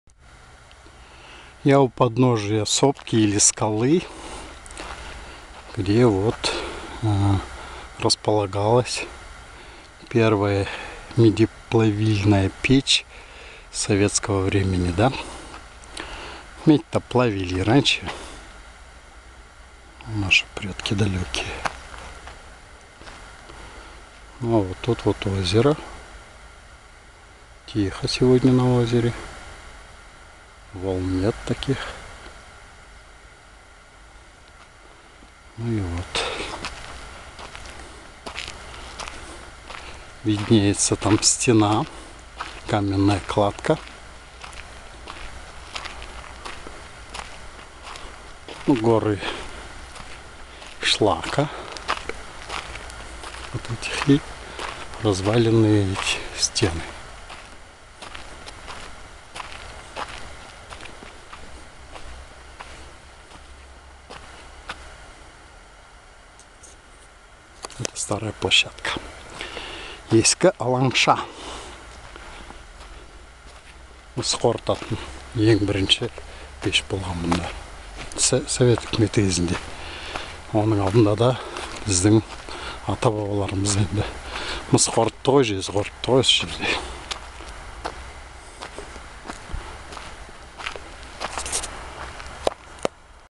Обзор вокруг у подножья сопки с трубой печи на конце пути к Старой площадке Балхаша
Наконец-то дошли до Старой площадки Балхаша. Обзор вокруг у подножья сопки, где находится труба первой медеплавильной печи советского времени. Справа берег озера Балхаш и само озеро. За озером трубы БГМК. Слева крыши дач и дорога на Шашубай (Озёрный). Старая площадка (Ескі алаңша) (Old playground) - так называется то место, где была построена первая медеплавильная печь в советское время. Наши предки плавили здесь медь (жез, мыс) ещё издавна. Так начиналась история современного города Балхаш (Балқаш), который стоит на берегу лимана (бухточки, заливчика) обрамлённого от основной части озера Балхаш мысом Бертыс (Бір Тіс) и полуостровом Белые Камни (Ақ Тас). Наши предки всегда плавили здесь медь (мыс, жез) - поэтому это место Старая площадка (Ескі алаңша) (Old playground) тоже является олицетворением понятия Ата Мекен (Обитель Предков).